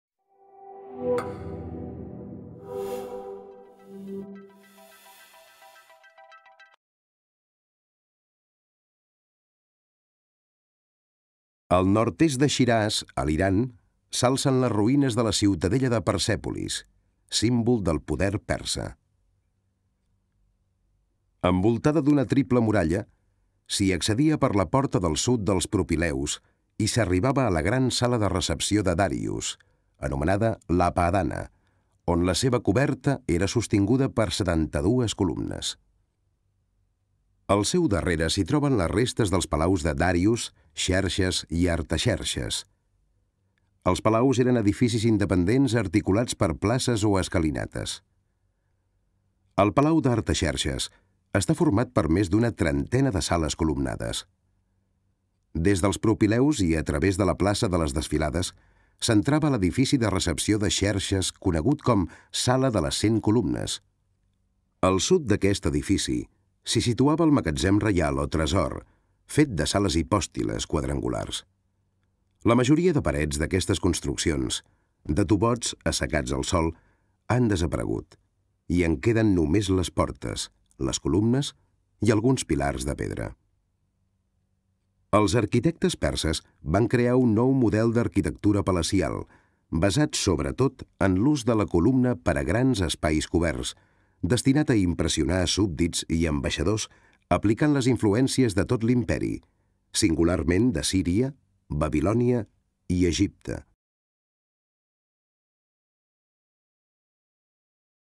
Resum d'Història de l'Art. 4. Persèpolis
És un breu itinerari per la ciutadella de Persèpolis que posa èmfasi en l’arquitectura palacial basada en l’ús de la columna, que recorre, des de la porta sud dels propileus, la sala de Dàrius (apadana), els palaus de Xerxes i Artaxerxes, la sala de les 100 columnes i els magatzems reials.